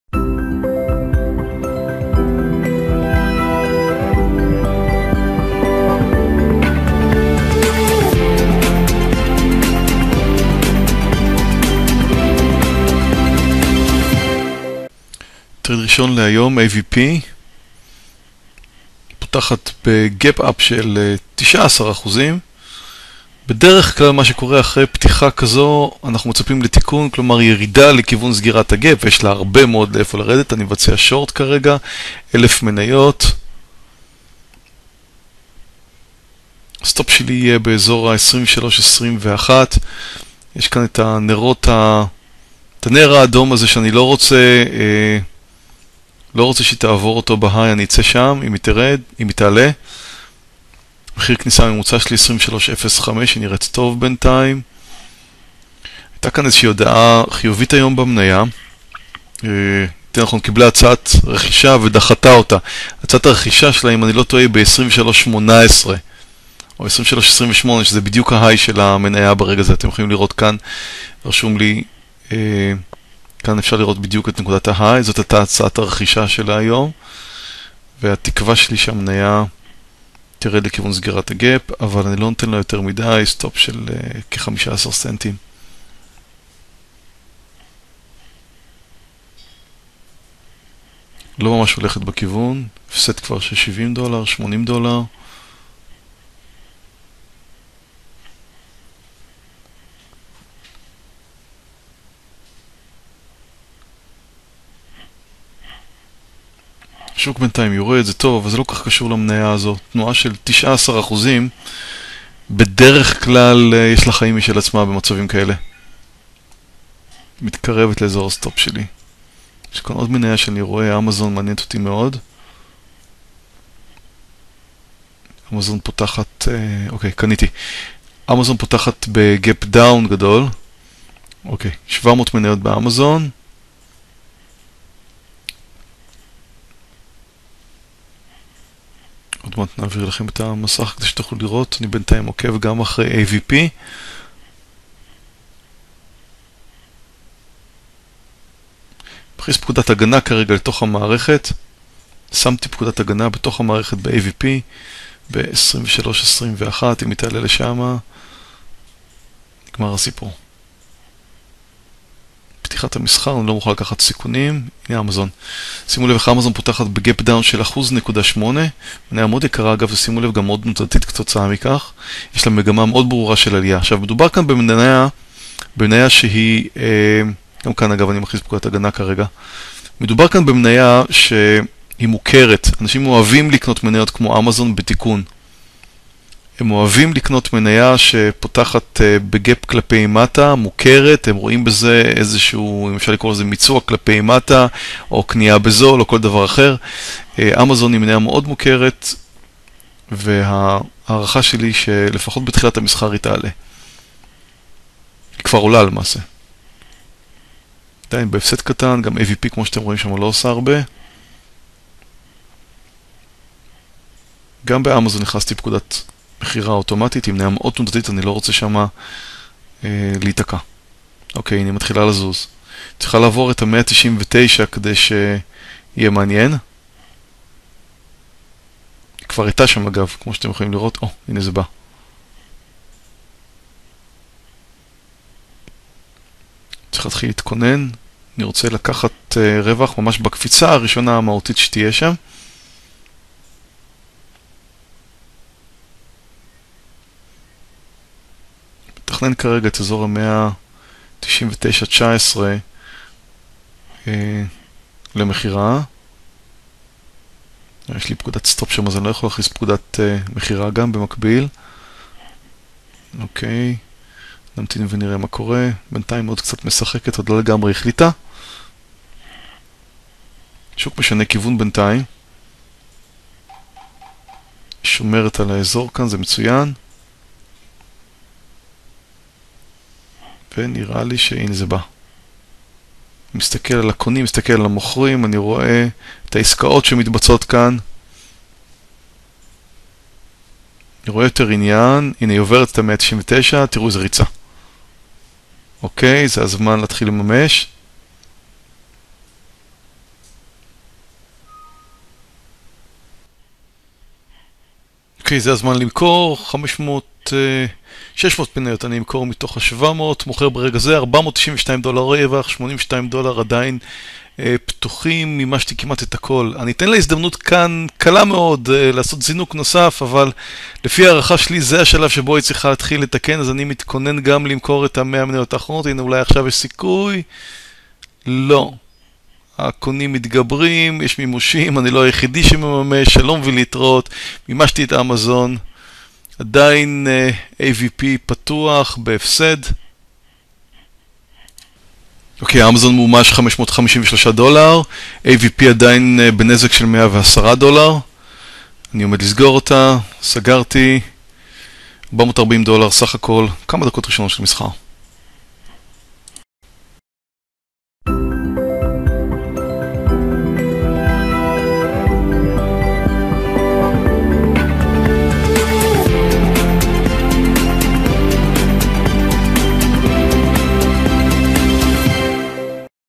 מסחר יומי - כמה אפשר להרוויח בעשר דקות? - מאיר ברק
מסחר יומי במניות בזמן אמת. איך טרייד אחד עושה את היום בעשר דקות. מאיר ברק סוחר במניות ומקליט את המסכים בזמן אמת. סרטון חובה לכל חובבי בורסה, משקיעים וסוחרי מניות. לימודי ומרתק! - יום מסחר: 26/03/2012. הצטרף לסדנת מסחר יומי עם מאיר ברק - בחינם! להרשמה